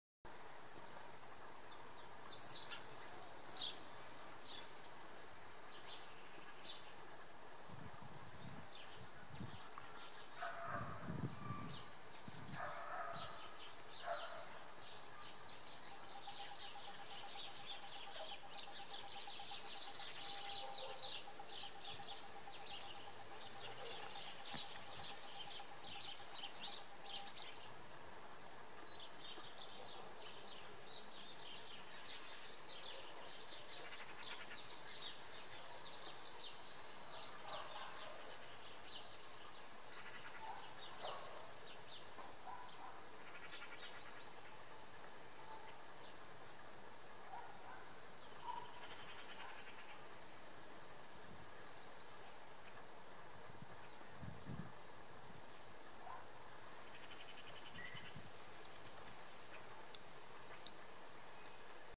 Nad ranem: szczygieł
Szczygieł w pochmurny poranek, na Oksywiu...